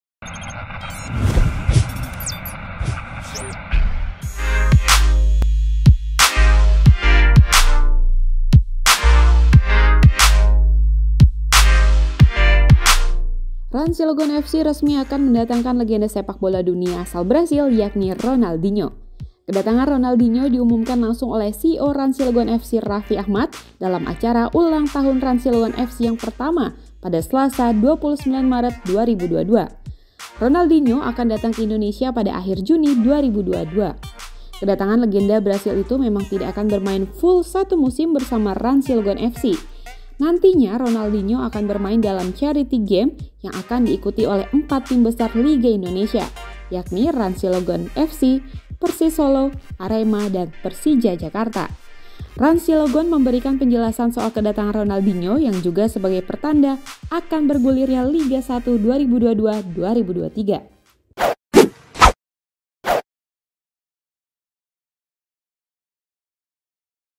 RANS Cilegon FC Resmi Datangkan Ronaldhino
Rans Cilegon FC resmi akan mendatangkan legenda sepak bola dunia asal Brasil, yakni Ronaldinho.